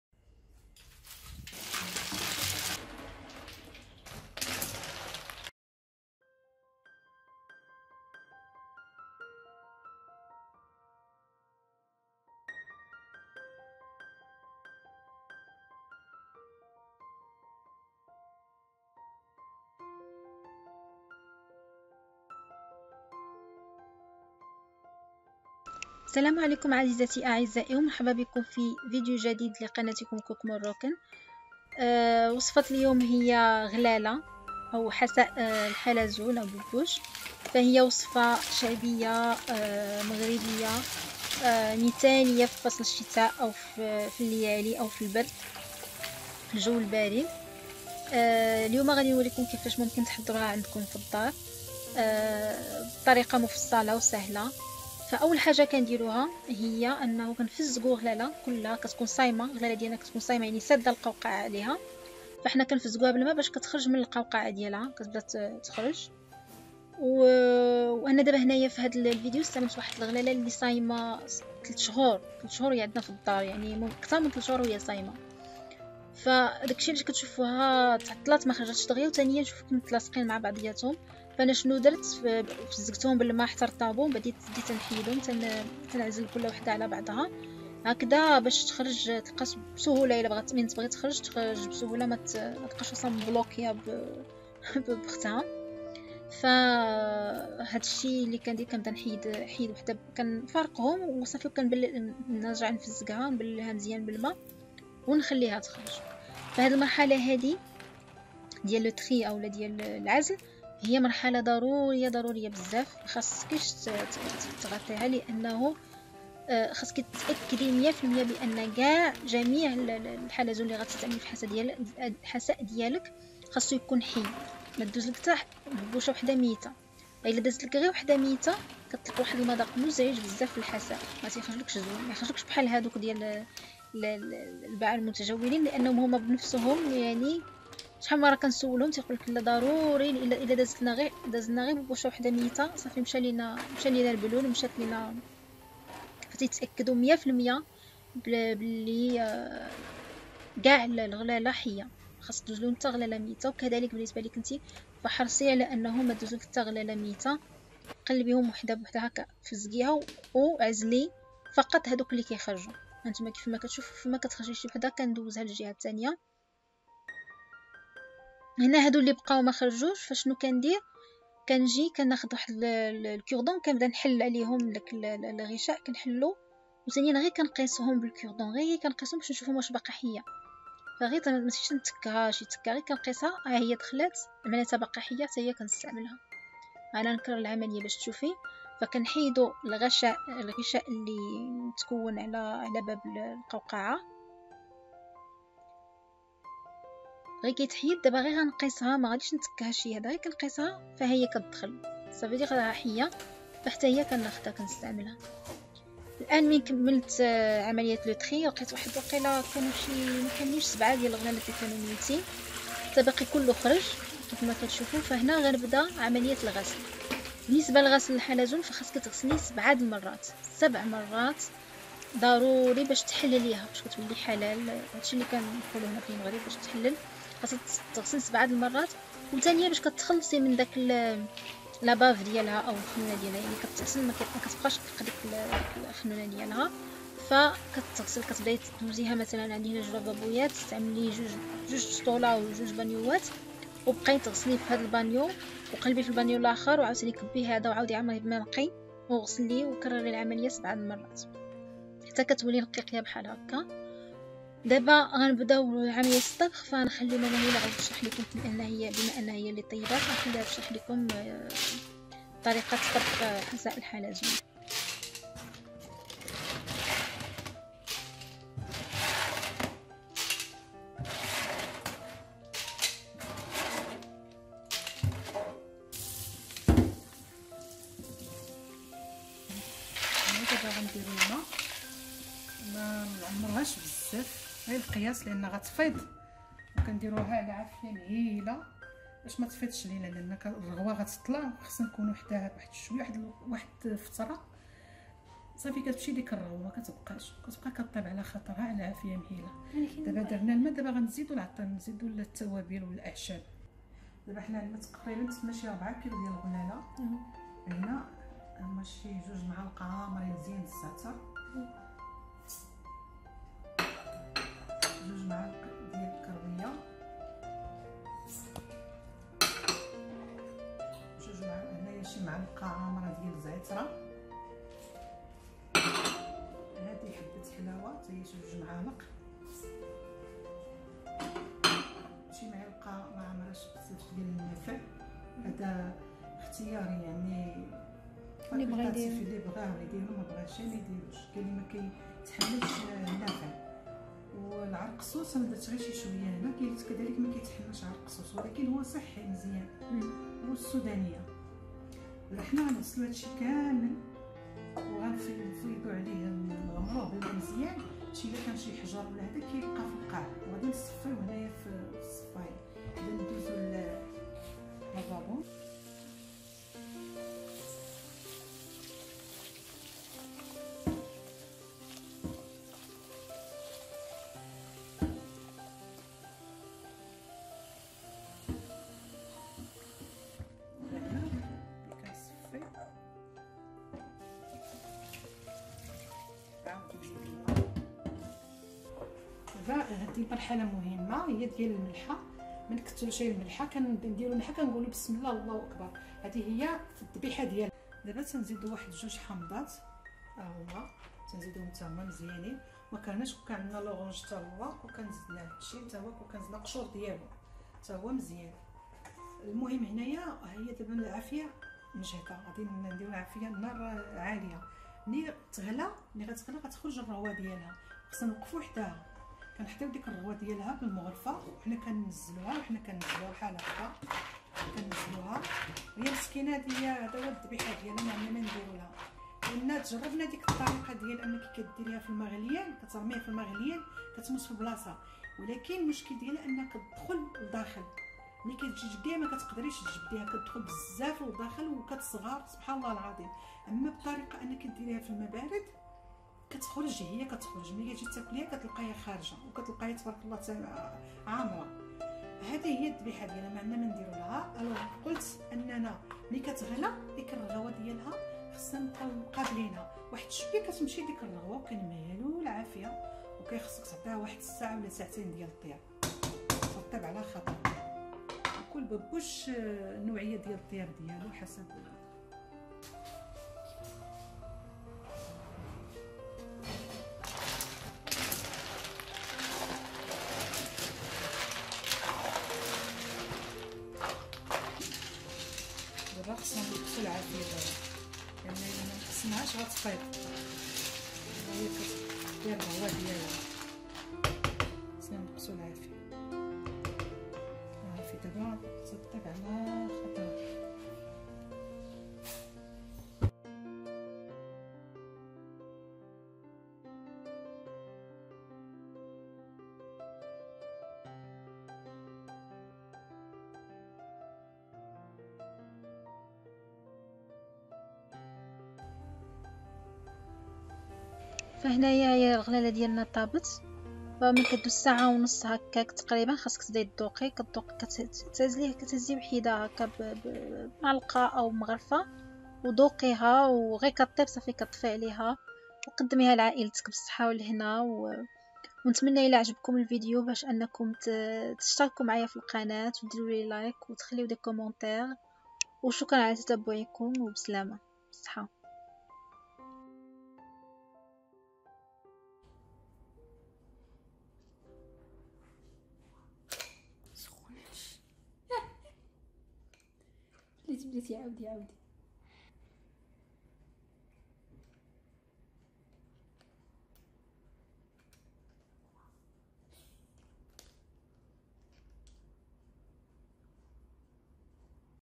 الطريقة المغربية المفصلة لغسل و تحضير حساء الحلزون أوغلالة في البيت
الطريقة المفصلة لغسل و تحضير غلالة أو حساء الحلزون على الطريقة المغربية في البيت

المقادير
ملعقتين كبيرتين حبة حلاوة أو حبوب شمر
عود صغير عرق سوس إختياري
ملعقتان كبيرتان زعتر جاف
ملعقة كبيرة زعيترة جافة
ملعقتان كبيرتان قعقلة أو حبة الهيل
ملعقتان كبيرتان كروية أو كراوية
ملعقة صغيرة نافع أو ينسون
فلفل حار جاف
ملح
قشر برتقالة و قشر ليمونة و قشر الرمان

ننشر صور ومقاطع فيديو إنستغرام  غير منشورة على اليوتوب .  لدي أيضًا معاينة للفيديو القادم، لذا يرجى متابعته

وبالصحة و الهنا 😘😘😘